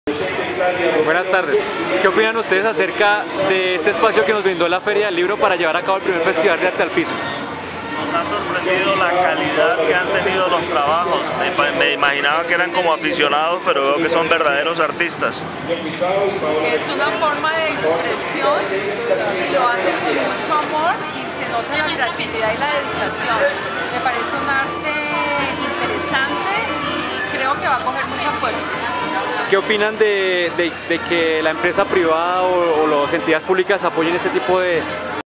Festival Arte al piso-Bogotá 2014
Respuesta del publico en el 1er Festival Arte al piso-Bogotá. Llevado a cabo dentro del marco de la XXVII Feria Internacional del Libro de Bogotá. (Corferias)